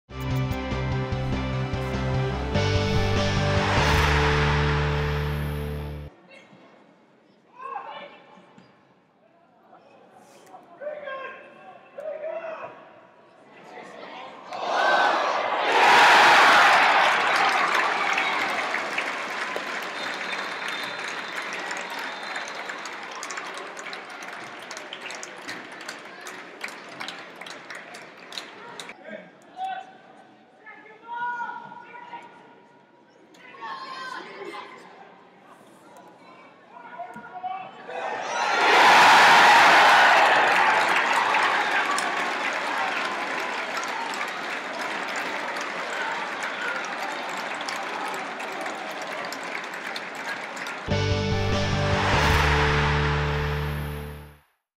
United U18s 2-1 Crewe - Blades goals
Goals from George Cantrill and Jordan Hallam saw the Blades U18s progress in the FA Youth Cup